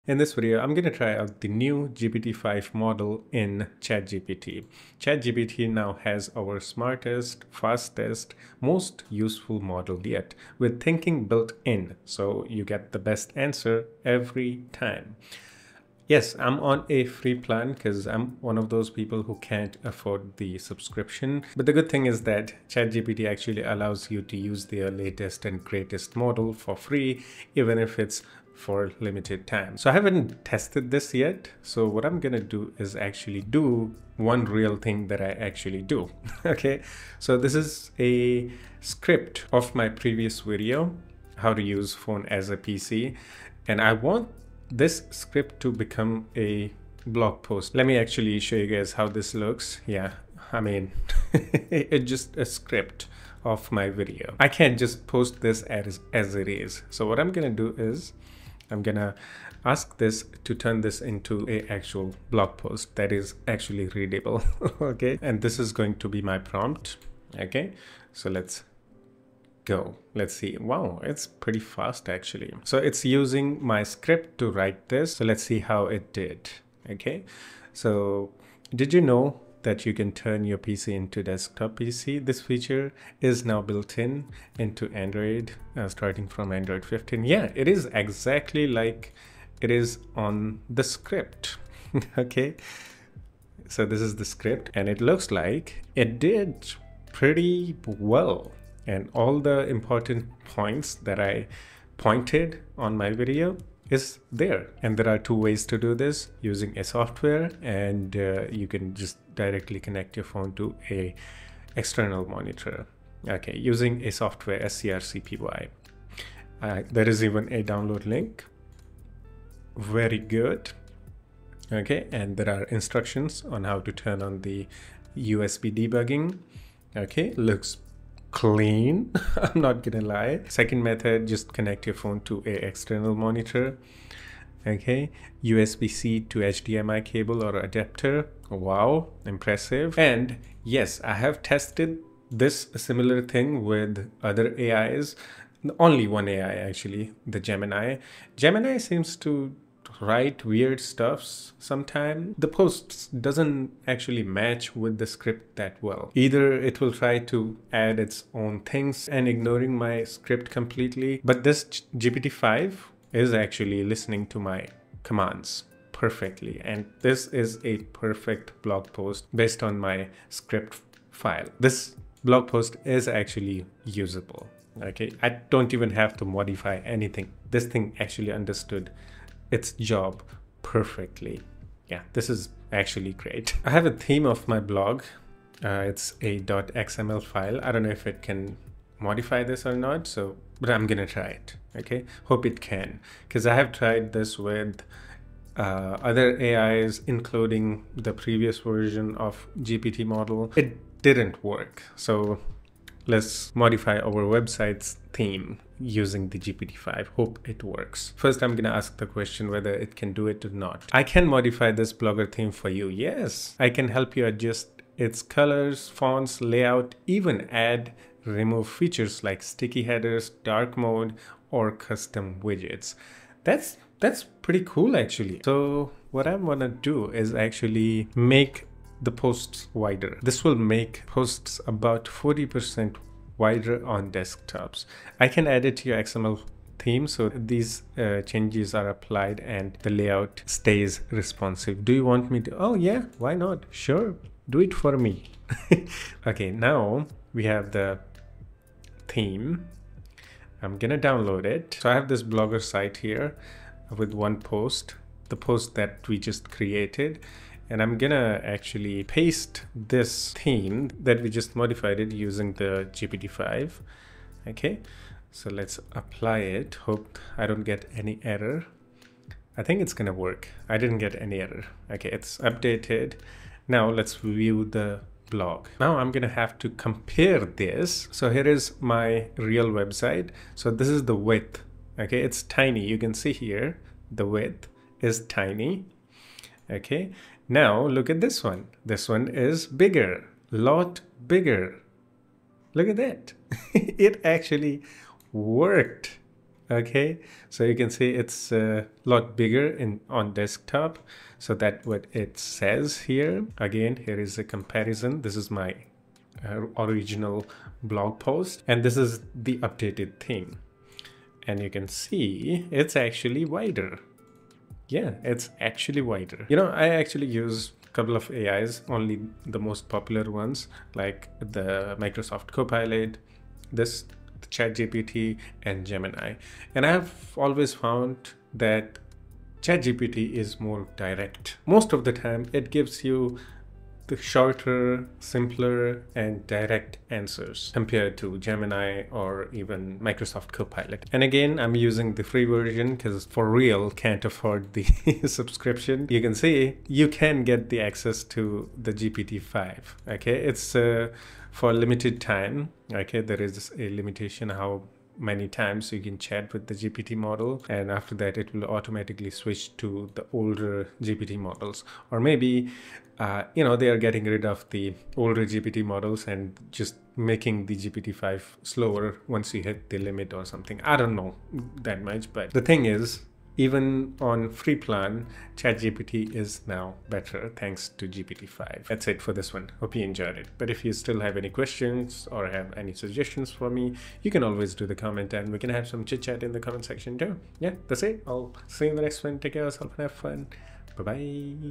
I Tried ChatGPT 5 and It’s Actually Smarter Than Ever
I tried out the new ChatGPT 5, and it’s actually smarter than previous versions!...